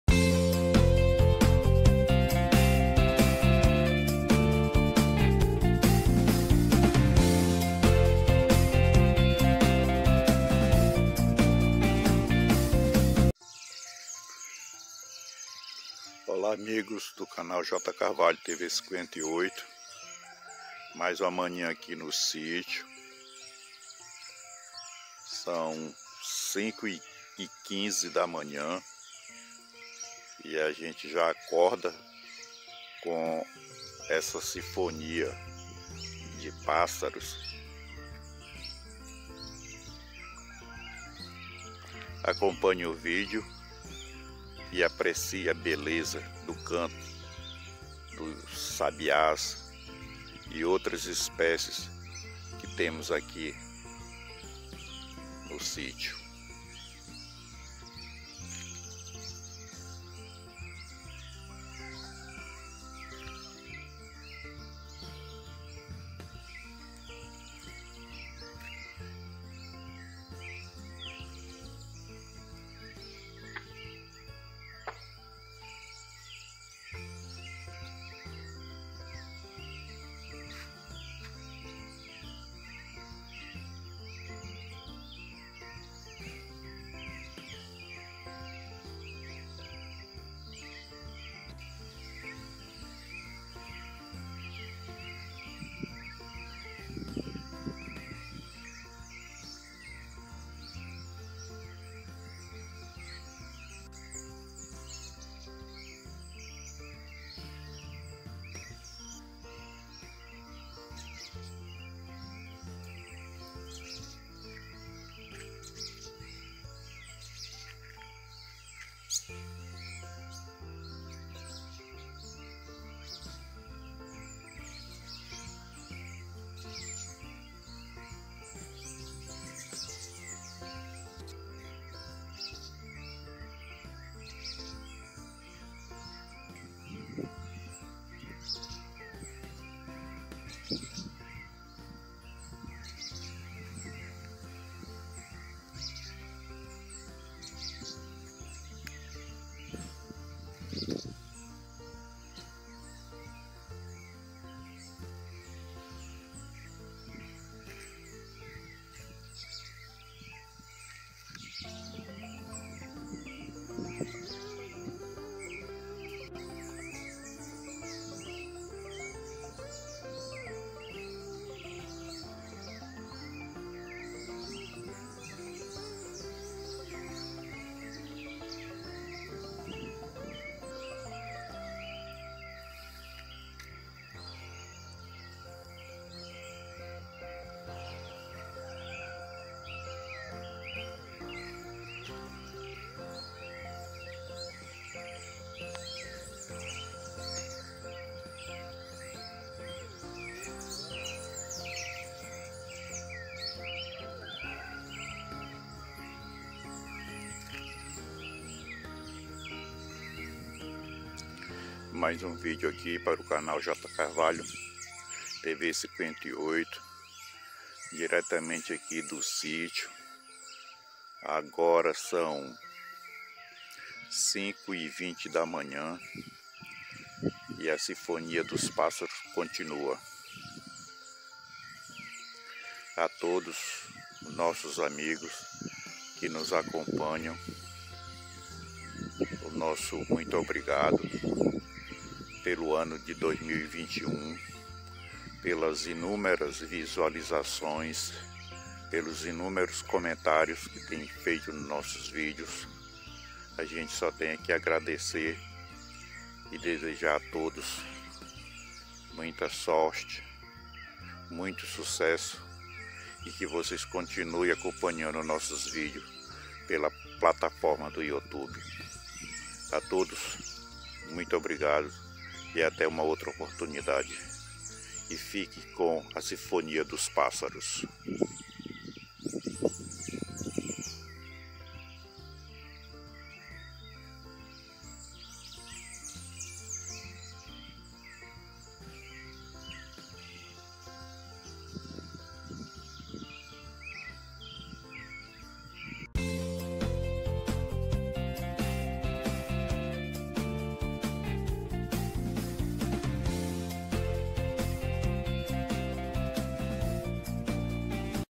"2021! A palavra é...GRATIDÃO!👋👋👋👋
Este vídeo mostra nosso amanhecer no sítio Pedacindo do Paraíso.
Gratidão!!!